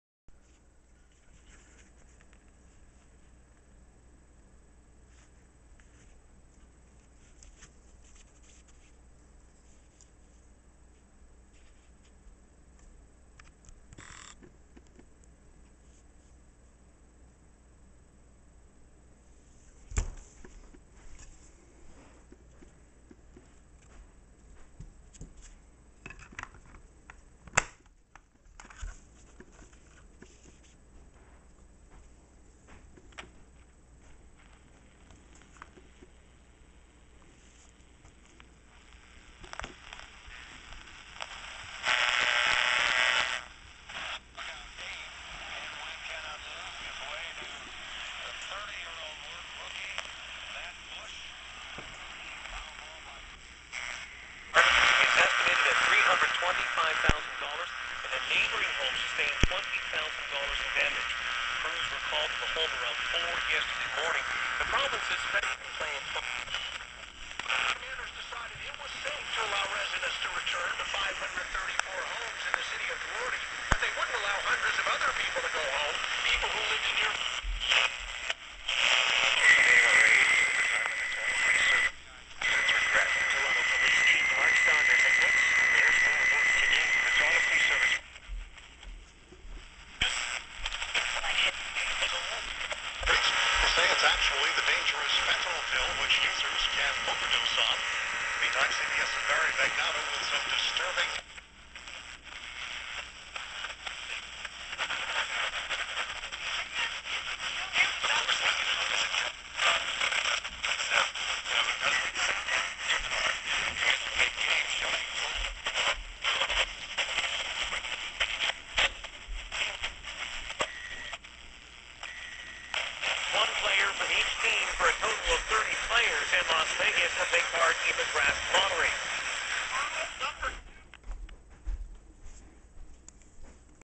My Hamilton Universal Six Transistor Radio in action after the restore!
This is a video of my Hamilton Universal Six Transistor AM Radio in action after the restore.